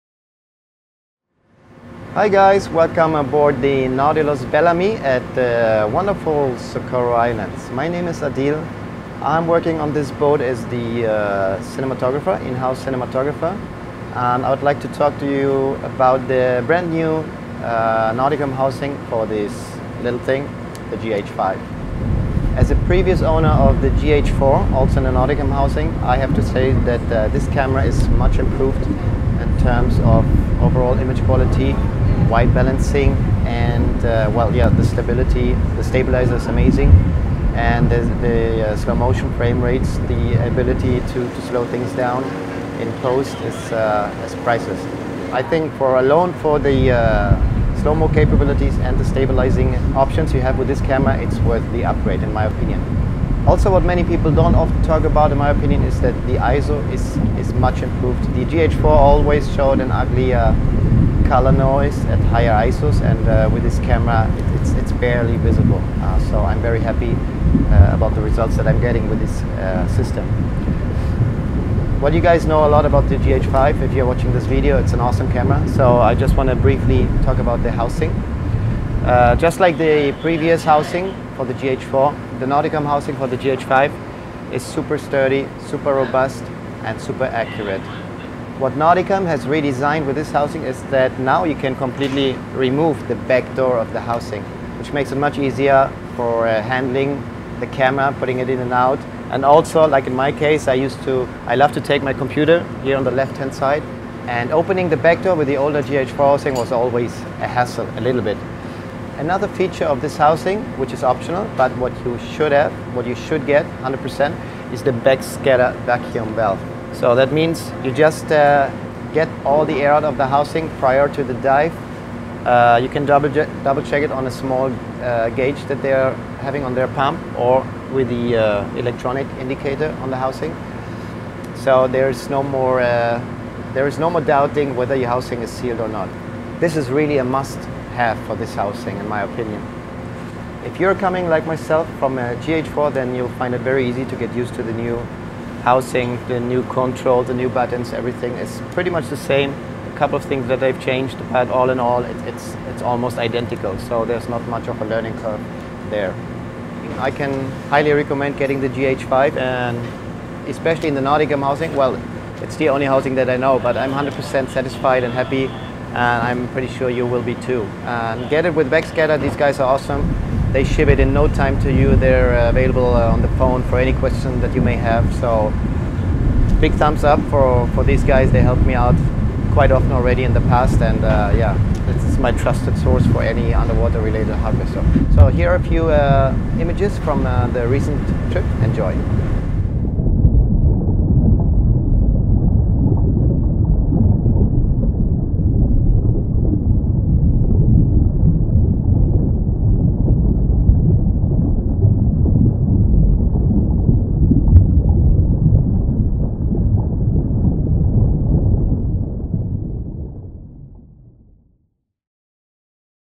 Panasonic GH5 & Nauticam Housing - Underwater Camera Review by Adil Schindler for Backscatter
Our good friend Adil Schindler of the Nautilus Belle Amie and Waterproof Video gives us his field-tested review of the Panasonic GH5 and Nauticam underwater housing. Adil put the GH5 through it's paces diving with the large animals of Socorro and breaks down the features that are most important for underwater shooters.